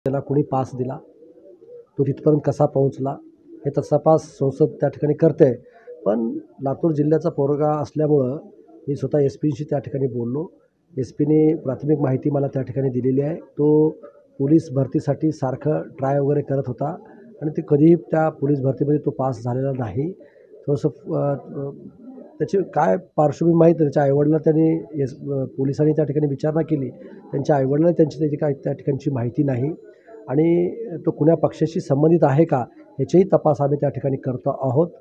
Sanjay Bansode : संसदेत घुसखोरी करणारा लातुरचा, पोलिसांकडून चौकशी सुरु
Sanjay Bansode : संसदेत घुसखोरी करणारा लातुरचा, लिसांकडून चौकशी सुरु